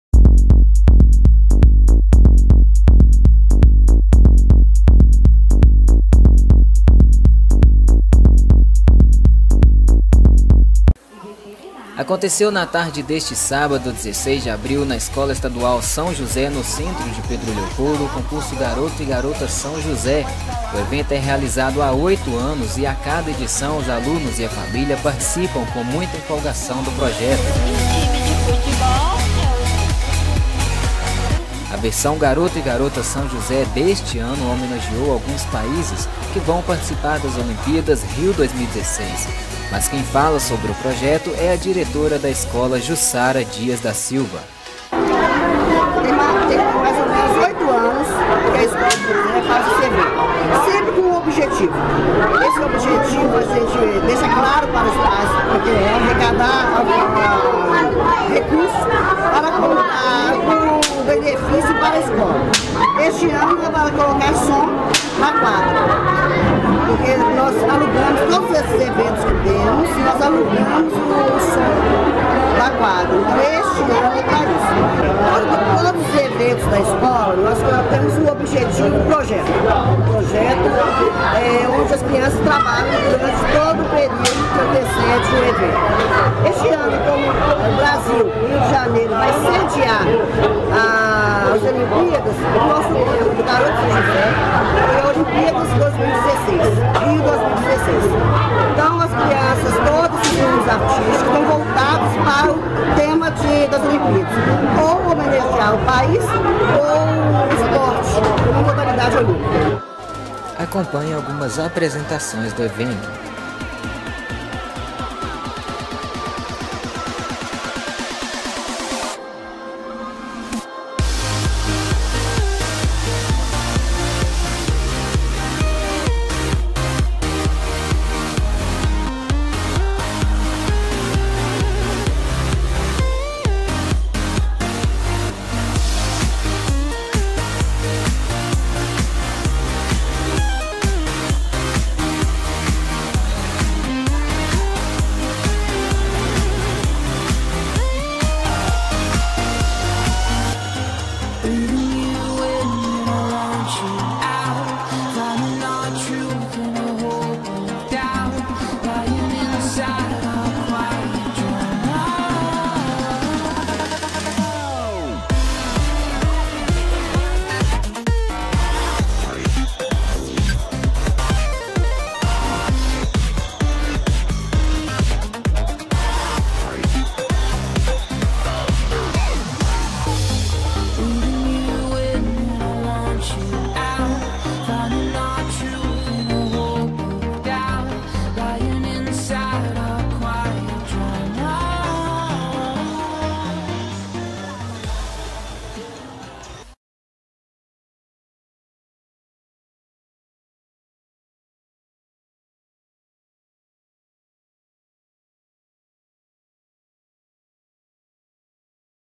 Concurso Garoto e Garota SÃO JOSÉ em Pedro Leopoldo
Escola Estadual São José, no centro de Pedro Leopoldo, presta homenagem a países que vão para as Olimpíadas 2016